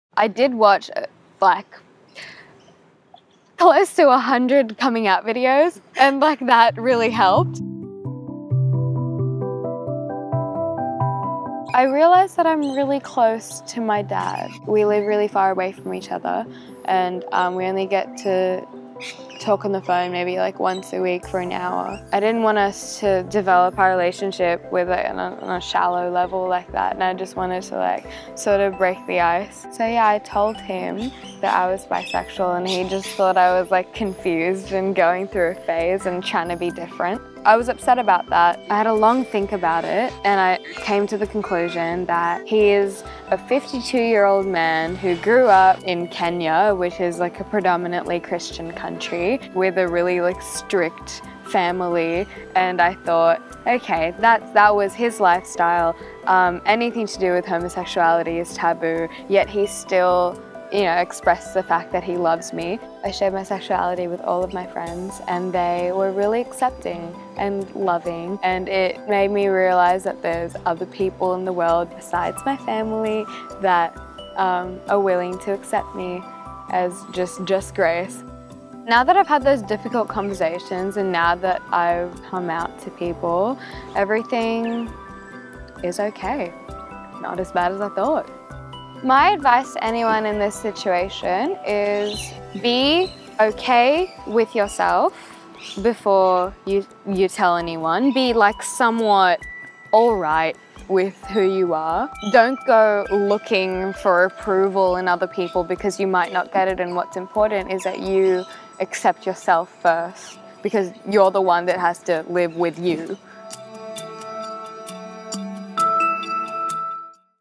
How Grace came out to her dad
While coming out to her dad, Grace found that self-acceptance was key.